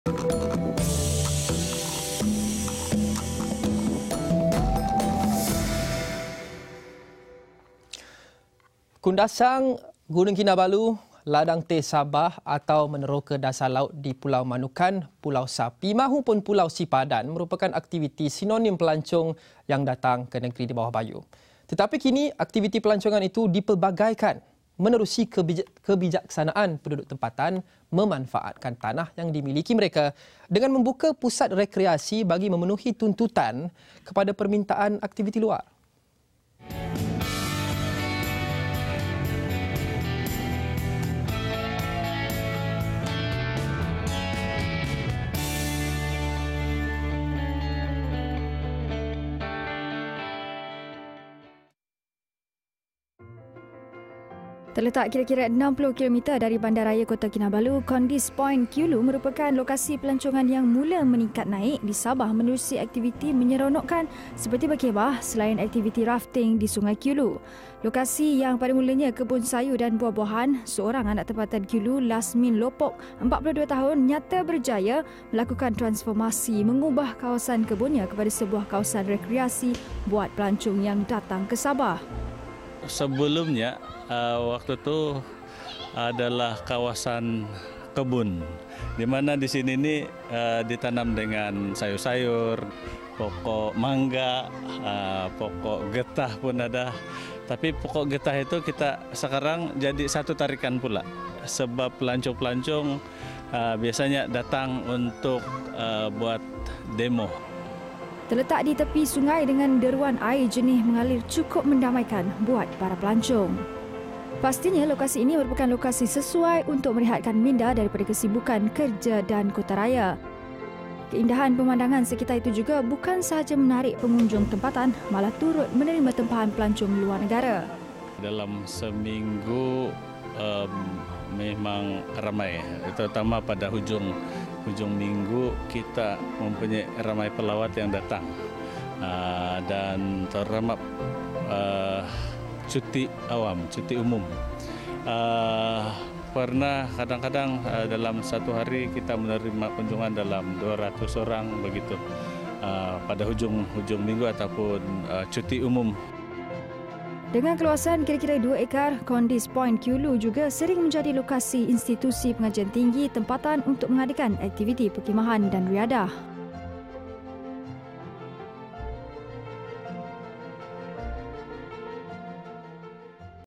Bicara Borneo: Momen luar biasa ditawar Kondis Point Kiulu
Kundasang, Gunung Kinabalu, ladang teh Sabah atau meneroka dasar laut di Pulau Manukan, Pulau Sapi mahupun Pulau Sipadan merupakan aktiviti sinonim pelancong yang datang ke negeri di bawah bayu.